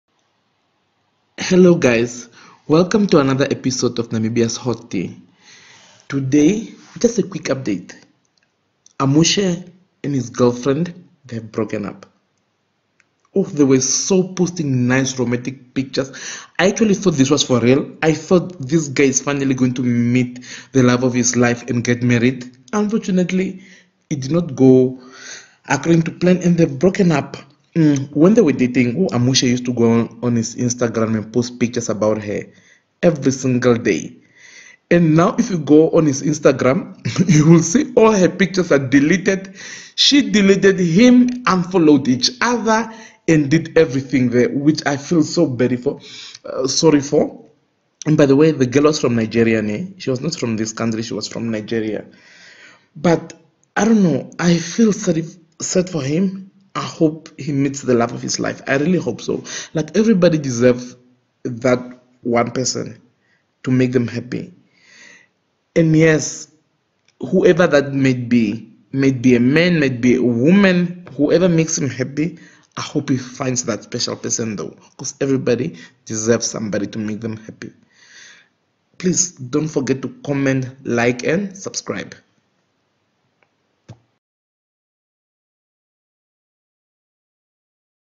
Michael Amushelelo and his Fiance have broken up
Hope he meets the love of his life

for any hot gossip